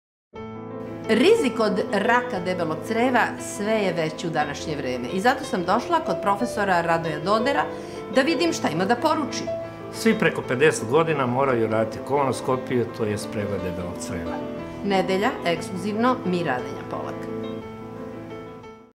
Mira Adanja Polak: Pregled koji spasava život – profesor dr Radoje Doder
Retko imamo priliku da neku intervenciju na koju nas pozivaju da je obavezno uradimo – vidimo kako se i izvodi. Upozorenja o raku debelog creva stalno su prisutna i podrazumevaju redovnu kontrolu, bez obzira na to da li ste u porodici imali nekoga ko je oboleo od te bolesti ili ne. Način života svakako igra veliku ulogu kao i neuredno pražnjenje creva, uz veći rizik ako imate i nasledni faktor. Postoji strah i od pregleda – gastroskopije, kada vam ubacuju crevo kroz usta kako bi videli jednjak, želudac i dvanaestopalačno crevo, taj put do želuca i sam želudac...